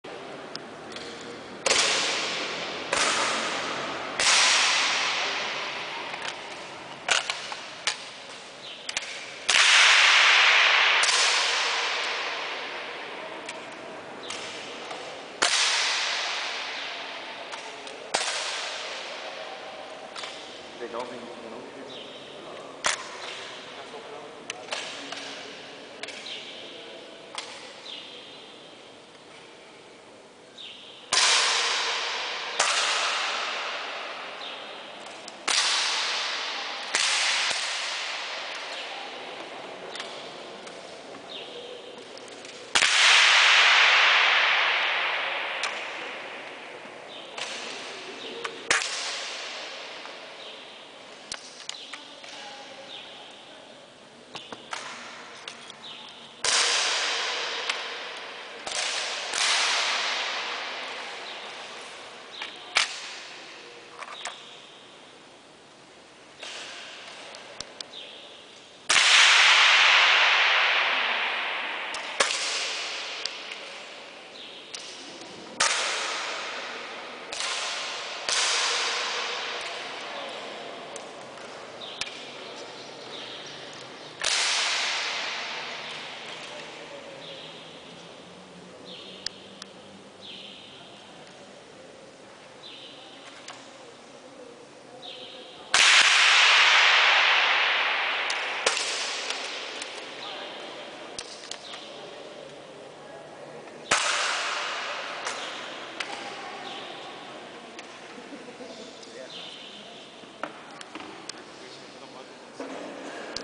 GATE São Gabriel-RS - 5ª Prova de tiro carabina de ar. 11-12-2010.
Prova de encerramento do campeonato 2010 de tiro esportivo realizada em São Gabriel - RS, modalidade carabina de ar promovida pelo Grupo Águia de Tiro Esportivo.